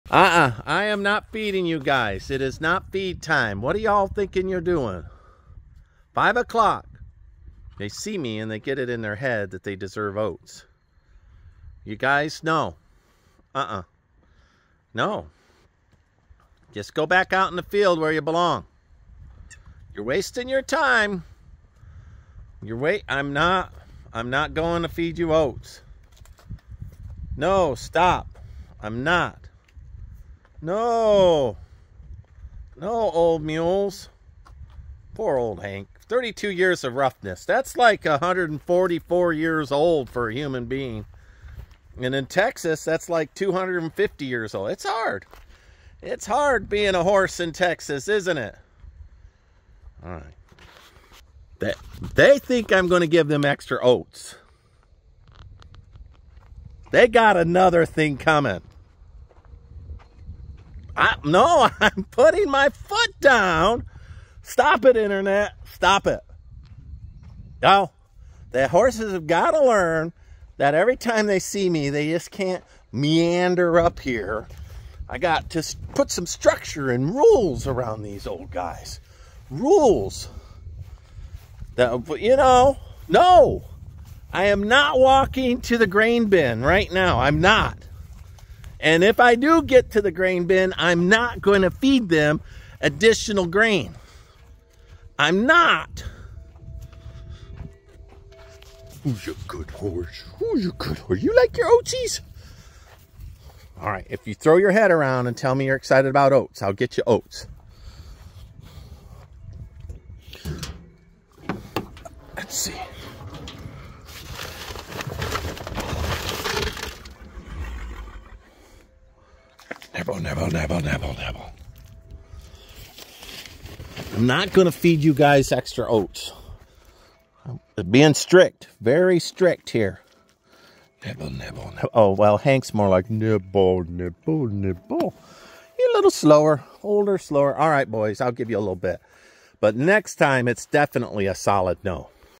Horses Jedi Mind Trick Owner
The horses hear me out and about and silently trick me into giving them a snack.